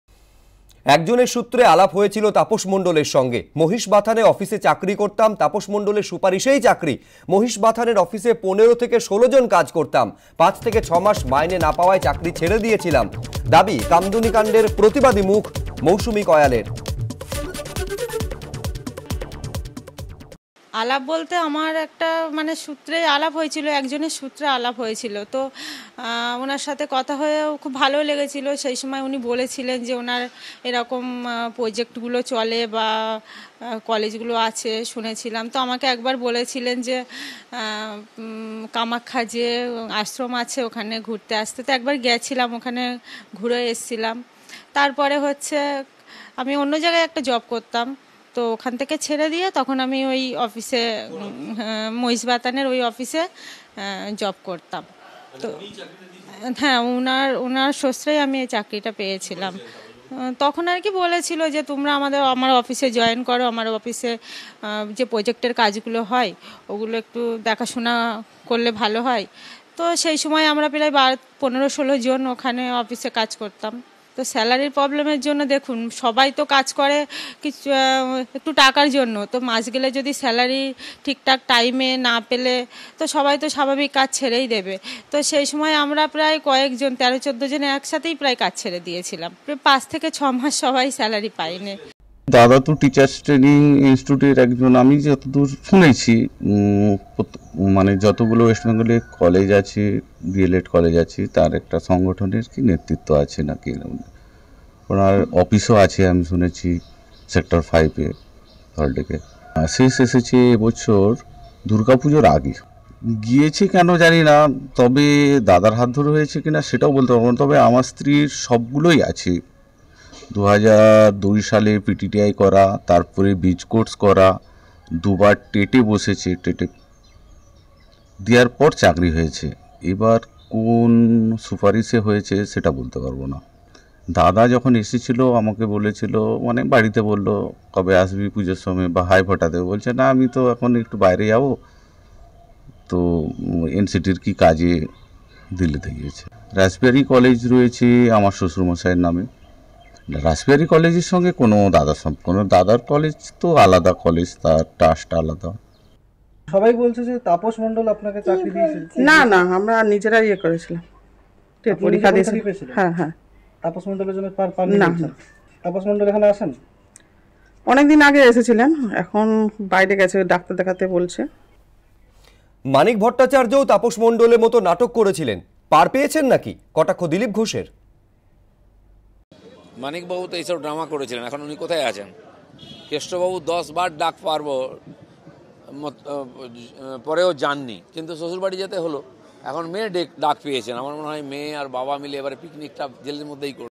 TET Scam: '৫ থেকে ৬ মাস মাইনে না পাওয়ায় চাকরি ছেড়েছিলাম', তাপস-মামলায় দাবি মৌসুমীর
'৫ থেকে ৬ মাস মাইনে না পাওয়ায় চাকরি ছেড়ে দিয়েছিলাম’, দাবি কামদুনিকাণ্ডের প্রতিবাদী মুখ মৌসুমী কয়ালের।